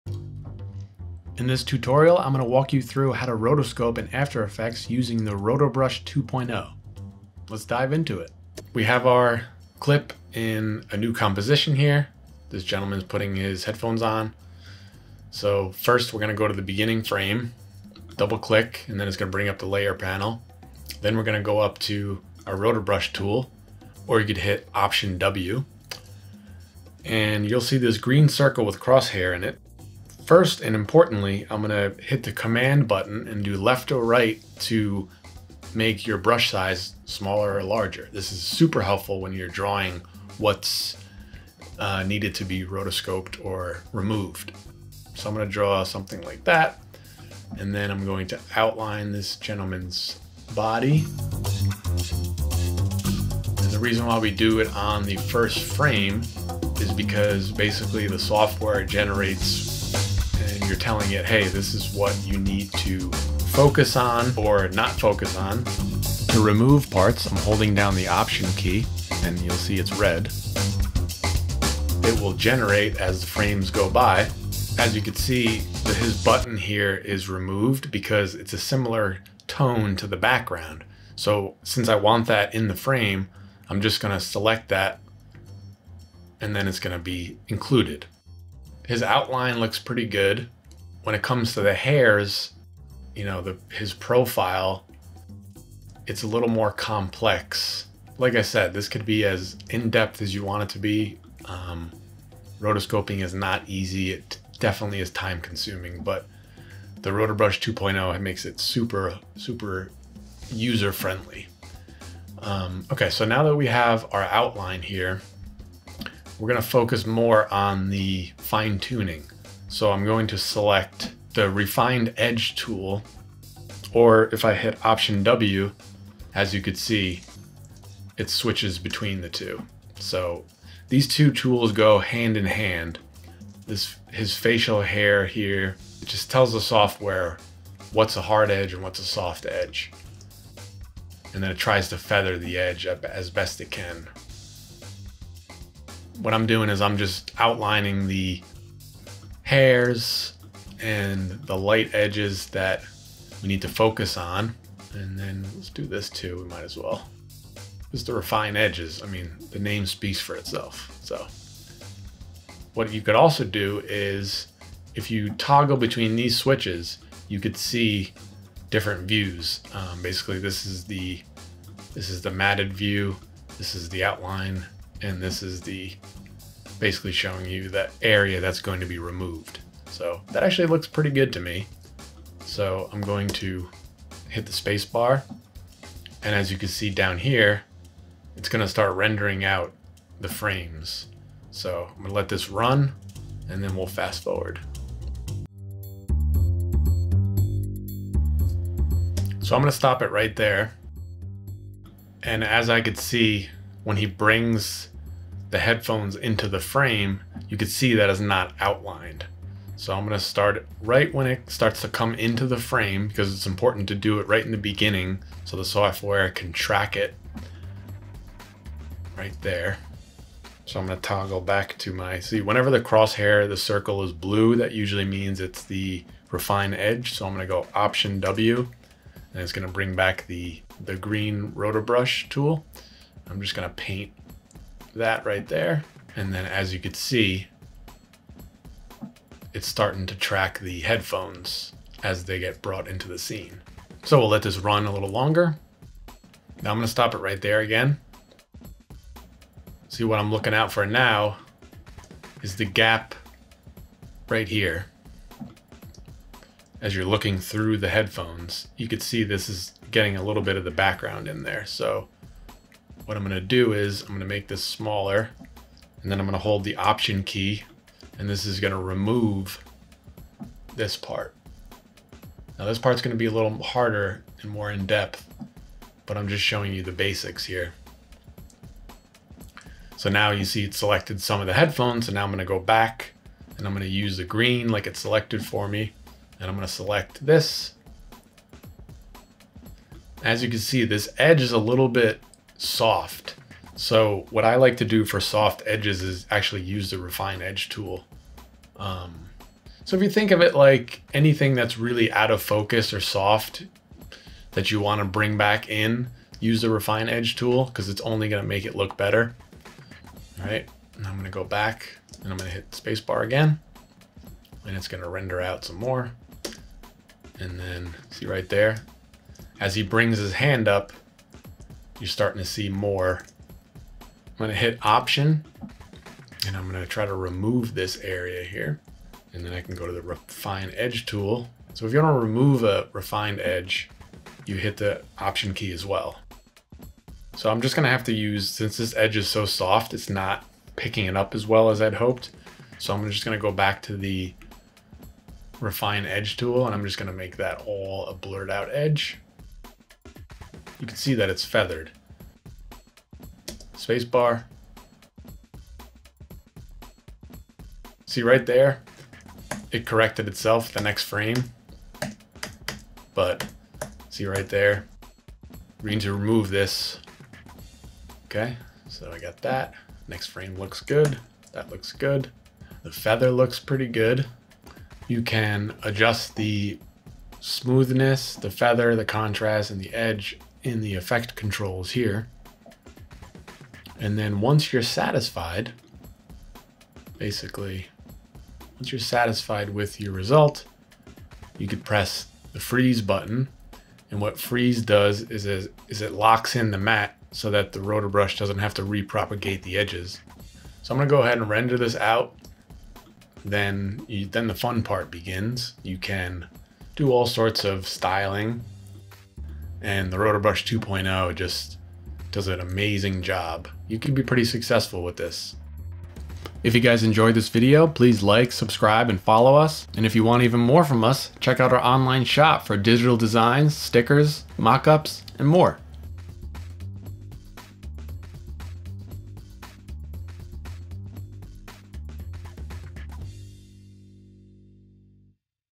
How to Rotoscope in After Effects Using Rotobrush 2.0 | Full Tutorial
Rotoscoping using Rotobrush version 2.0 is really simple, but it can be intimidating if you've never done it before. But in this tutorial I demonstrate the basics of rotoscoping, and how easy it can be. This tutorial assumes you have a basic understanding of After Effects, and have knowledge of layers, compositions, and simple effects.